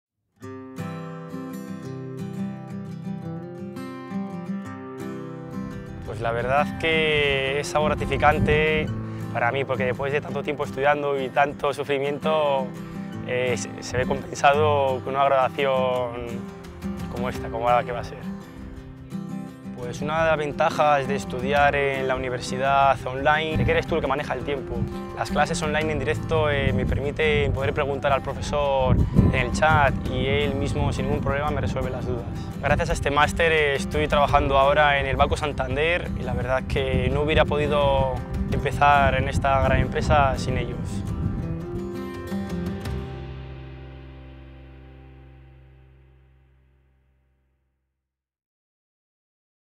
Testimonio de Jorge Hernáez, alumno del Máster MBA, promoción de 2015
Jorge Hernáez nos cuenta su experiencia estudiando en UNIR el día previo a su graduación.